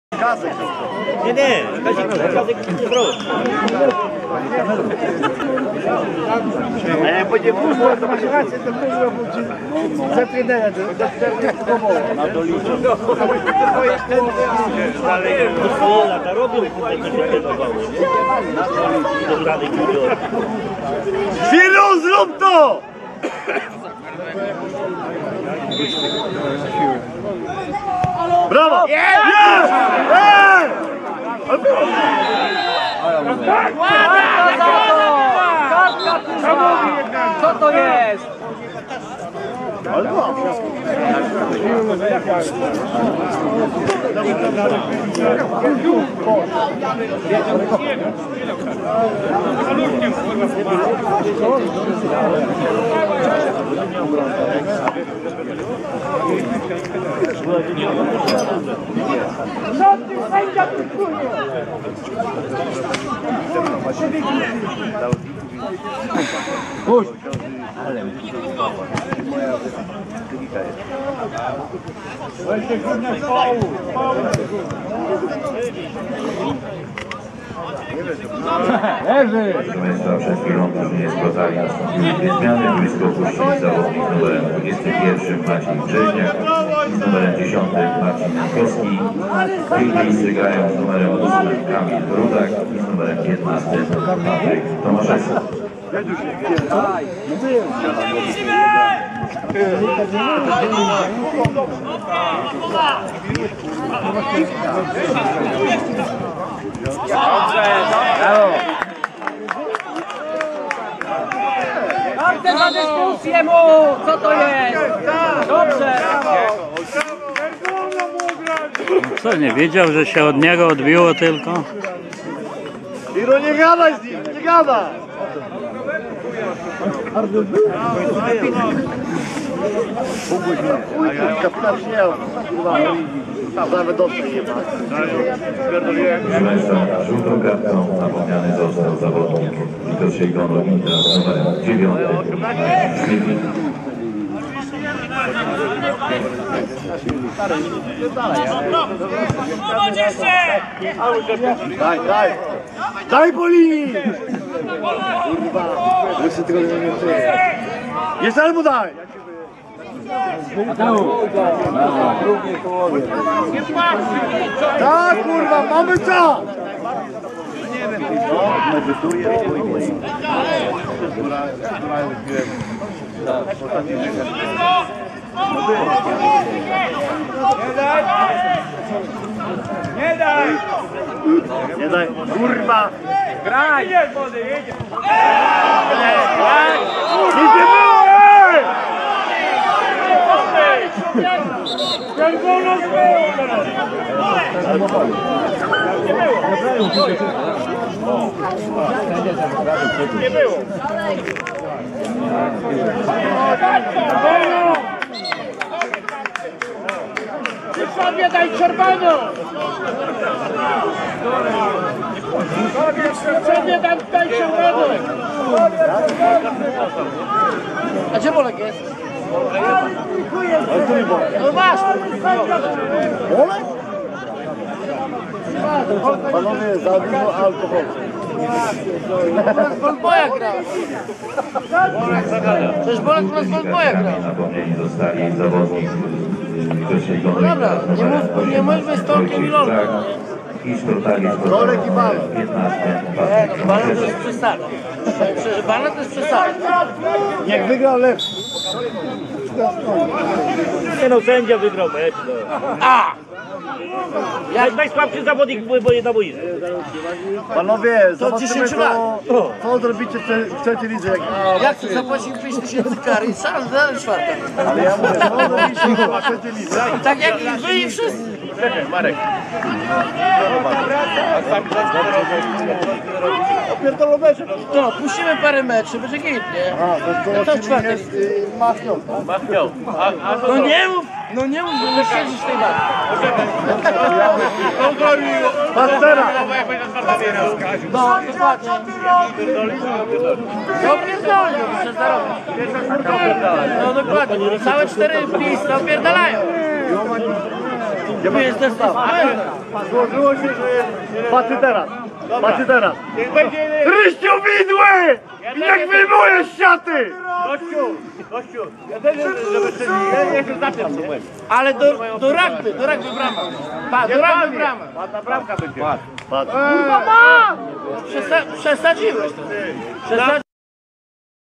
5.11.2011 r Vitrosilicon-Intra Iłowa : Sprotavia Szprotawa .5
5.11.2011 r Stadion Miejski     Iłowa.   Mecz IV -LIGI  Vitrosilicon-Intra Iłowa  :Sprotavia Szprotawa    0 : 1
.Bramka .:  Jankowski  Marcin  18 min.

.   .SPROTAVIA (zielono -czarne)
. Skład : bra.(1)Korotyszewski  Marcin ,kap.(13) Czekurłan  Tomasz ,(7) Chmielewski Paweł ,
.(14) Galusinski  Przemysław ,(5) Jagiełlo  Arnold ,(3) Nowicki Kacper , (21)Brzeżniak   Maciej ,
.(16) Bigus Paweł ,(2) Tomczyk Wojciech ,(10) Jankowski Marcin ,(17) Aksamitowski  Wojciech .
.  Rezerwa : (11) Gonciarz Łukasz , (19) Kwiatkowski Kamil ,(20) Jarosz Rafał ,
                 (8) Rudak Kamil , (15) Tomaszewski Patryk .
 Trener Sobczak Mieczysław .
.  Kierownik Rajkiewicz Stanisław .
      Vitrosilicon Intra (niebieskie)
.   Skład :  br.(12) Stopekura Łukasz  kontuzja     `46" ,   (1) Kowalski Manuel   (2) Nems Kamil ,
               (6)  Bagiński Andrzej  (3)  Kasperczak Jarosław   ,(8) Pluta Krzysztof  ,
                .(5) Rakowski Grzegorz  zmiana 67",  (15) Dobroliński Łukasz   , (10) Koseła Maciej  .
               .(18)  Wojciech Frach   , (13)Brzezicki Bartosz  ,(19)  Łojko Marcin  , (9) kap.  Świrlik Marcin
.    Trener: Sobczyk  Artur.
.   Kierownik : Radomirski Janusz .